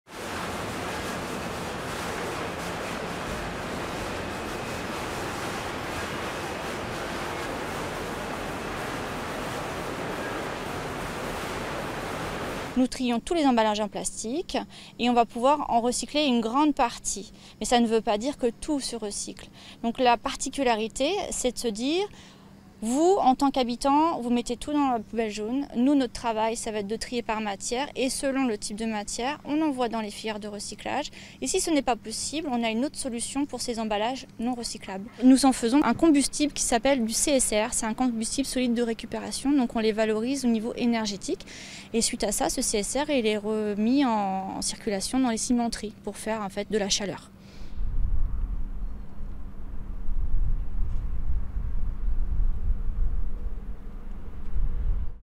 En Bretagne, une usine pionnière sur le tri des plastiques
Traiter les plastiques usagés en visant l'objectif de 100% de plastique recyclé voulu par le gouvernement à l'horizon 2025: le syndicat de collecte des déchets Kerval le fait déjà depuis quatre ans en Bretagne. Durée: 00:55